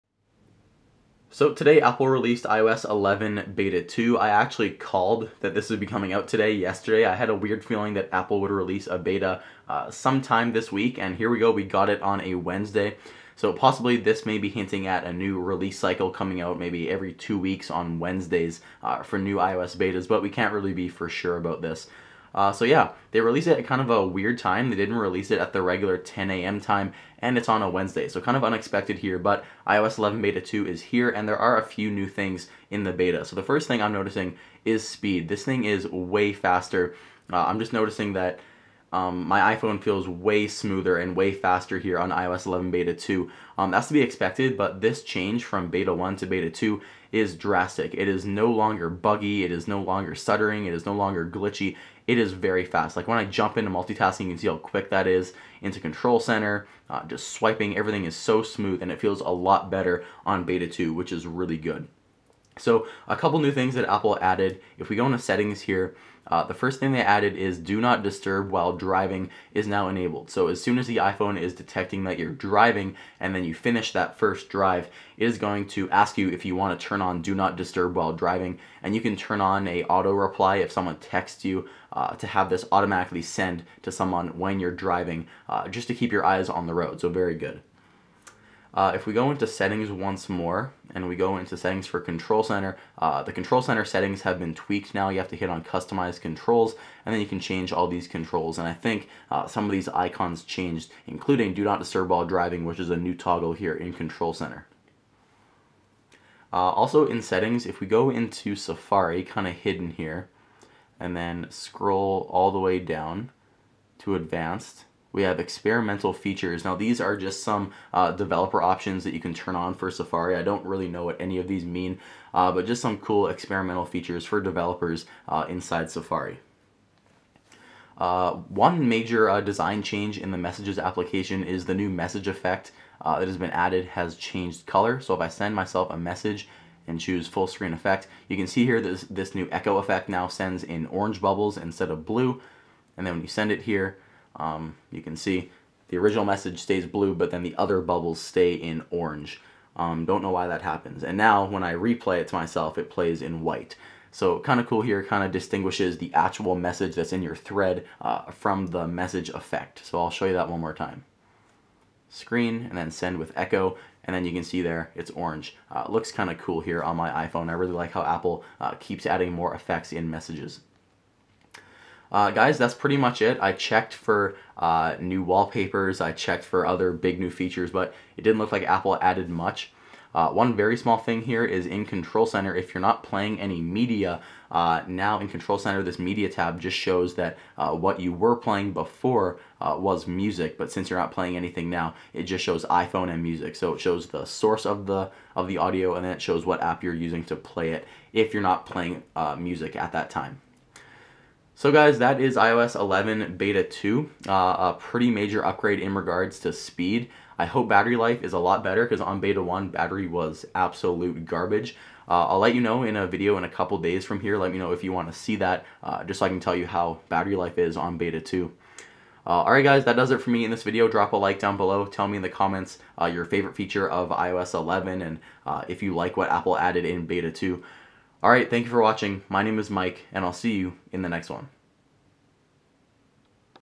iOS 11 Beta 2 RELEASED! New Features & More!
iMyFone Umate Pro can delete junk, memory hogs and cache to give your iPhone a spring-clean speed boost and get it running faster.
iOS 11 Beta 2 is here and Apple has made some great improvements. Do Not Disturb while driving is now enabled to keep you safer when behind the wheel, there are new colours to iMessage effects, and overall performance has been heavily upgraded.